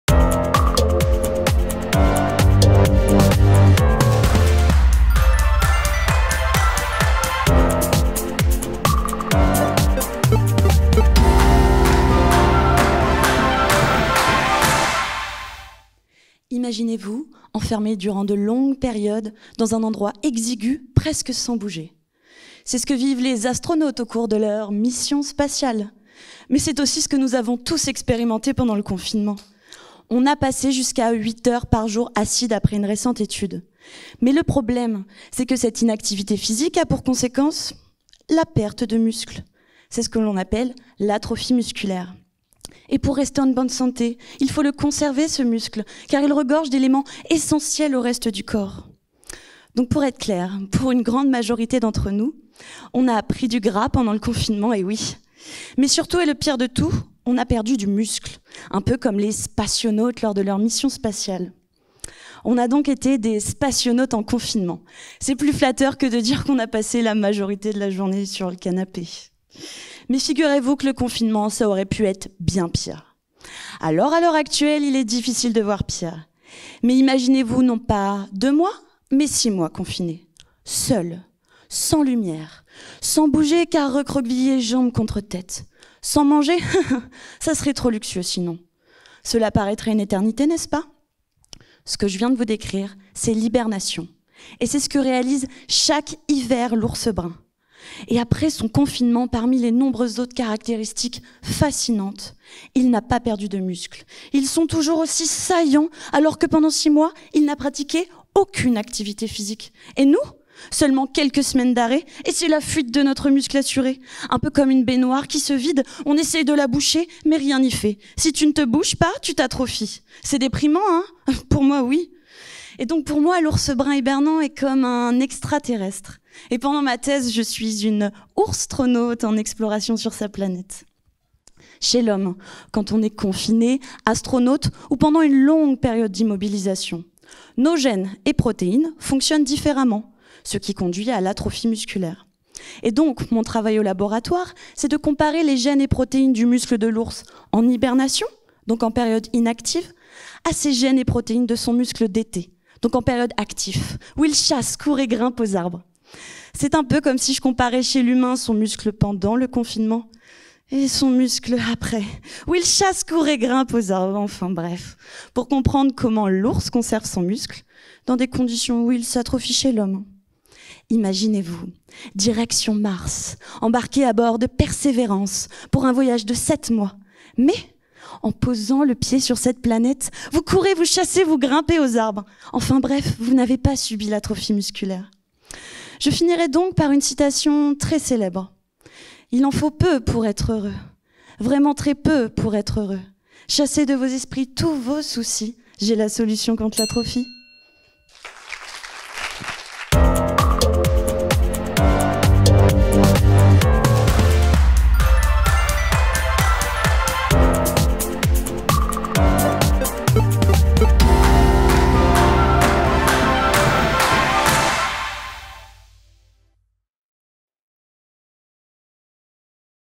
Ma thèse en 180 secondes (2021) - Laura CUSSONNEAU (prix des internautes et 2e prix du jury)
Ma thèse en 180 secondes // Edition 2021 // Clermont-Ferrand
Laura CUSSONNEAU
Thèse : Comprendre les mécanismes impliqués dans la résistance à l’atrophie musculaire : l’intérêt du modèle de l’ours brun hibernant dans la lutte contre l’atrophie musculaire
Laboratoire : UNH
Université Clermont Auvergne

Réalisation : Direction de la communication de l'UCA